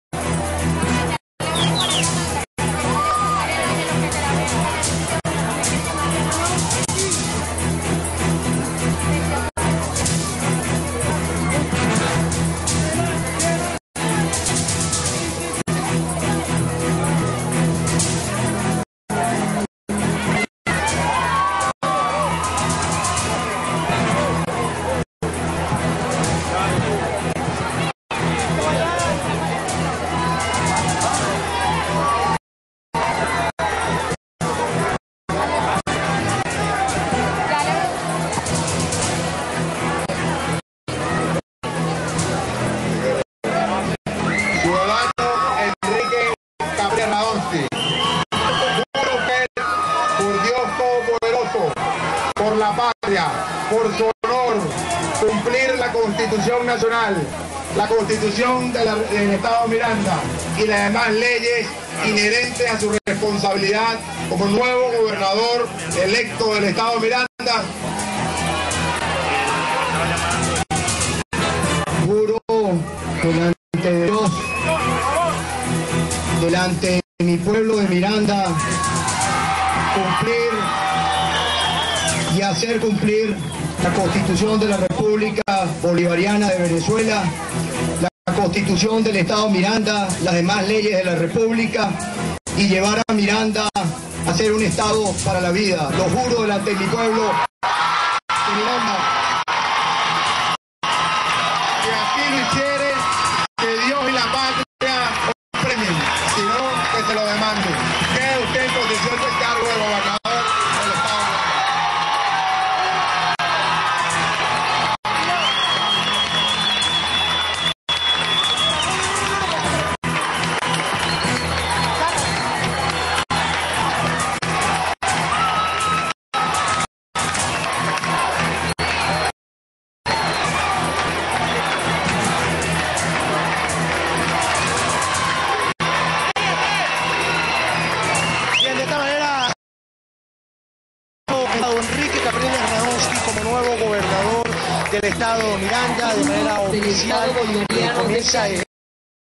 toma de posesión de Capriles Radonski.flv
Momento en que el nuevo gobernador del estado Miranda toma posesión de su cargo